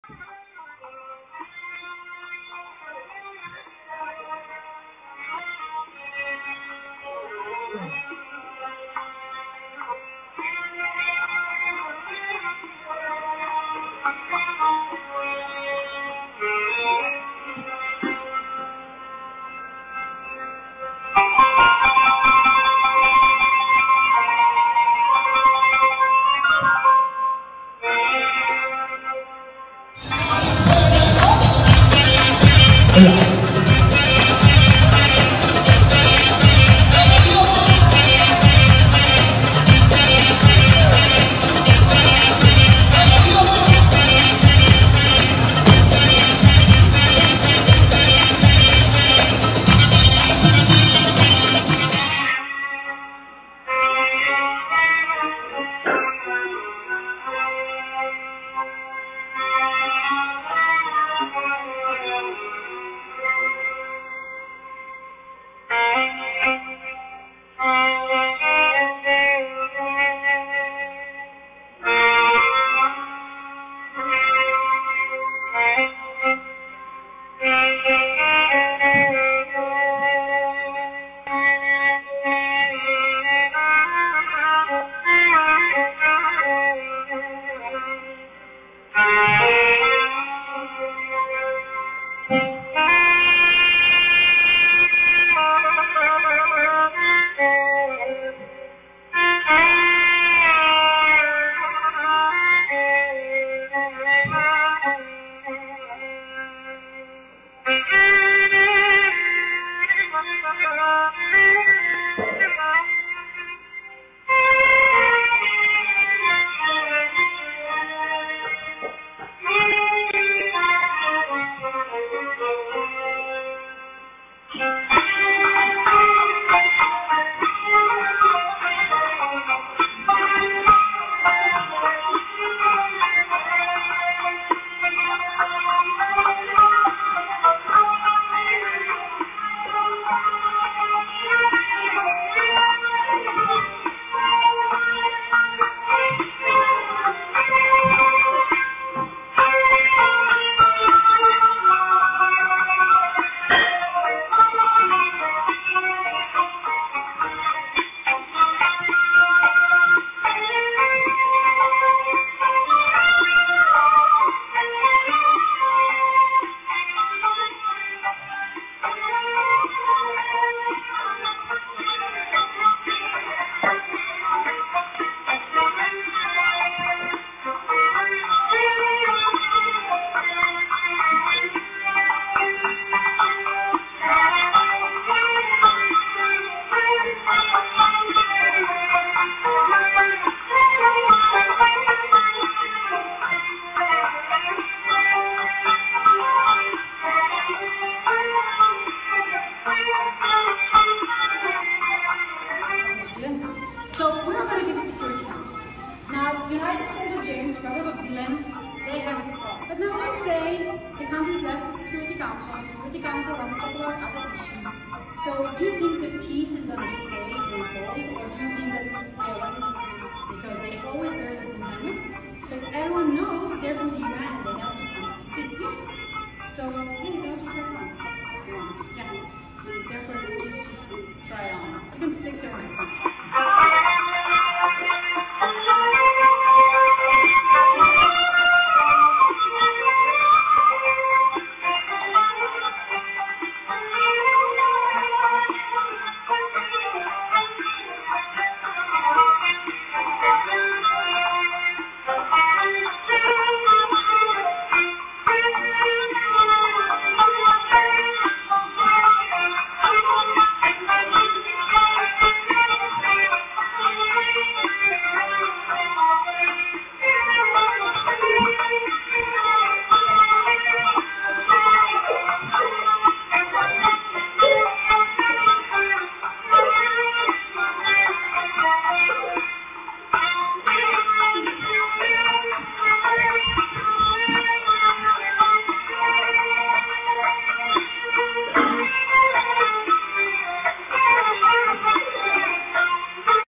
James Rada NewsCast: Visiting the United Nations Headquarters at NYC
My dad and I went inside the United Natikns Headquarters in NYC.

I Had a fun and learn day inside the United Nations Head Quarters. Currently there are 193 UN member nations. During our visit, the UN HQ is celebrating the Arabic Written Language so we watched an Arabic ethnic dance. (Hence the ethnic Arabic music background of this video).The general assembly hall where all nation leaders able to speak and address the entire UN members is quite large and impressive. We also learn about the 17 problem priorities of the UN that it is trying to solve. We also learned about the plight of refuges, environmental pollution, and the results of war. James is able to try the UN peacekeeping helmet or simply known as the blue helmet and he wants the kids version from the gift store.